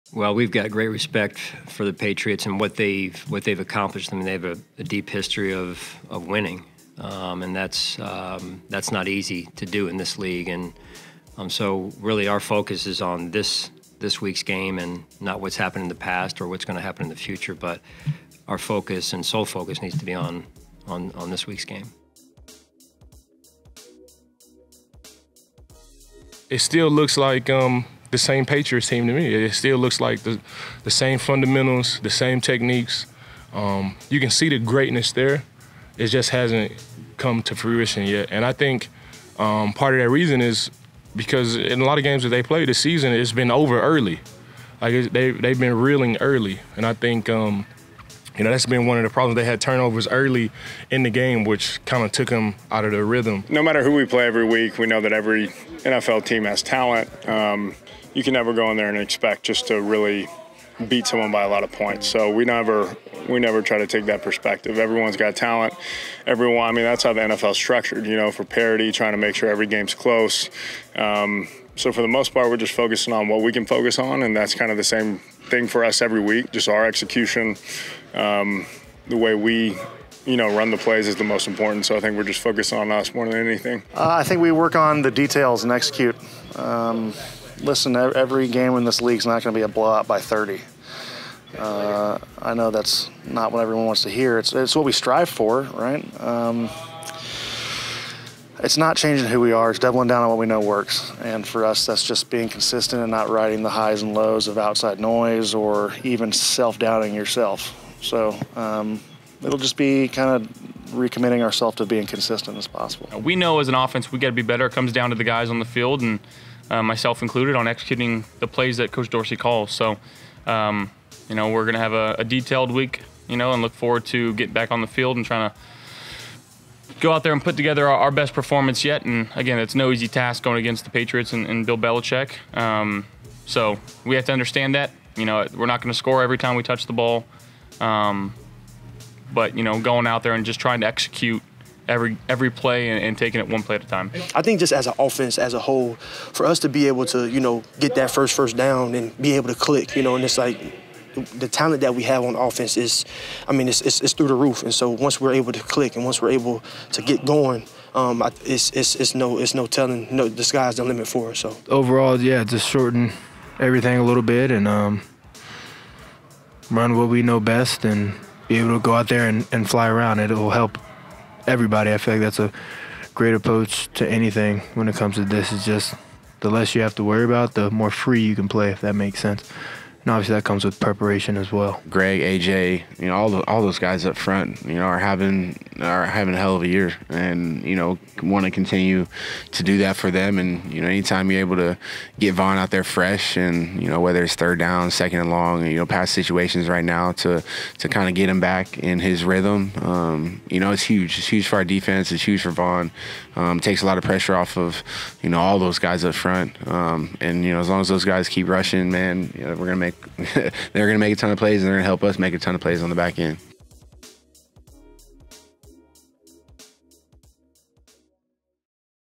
Buffalo Bills QB Josh Allen Vs Belichick's Defense | Top Sound Bits!
Hear some of the top quotes from Head Coach Sean McDermott, QB Josh Allen, and Bills players ahead of their game against the New England Patriots.
Sound Bits is presented by Tim Hortons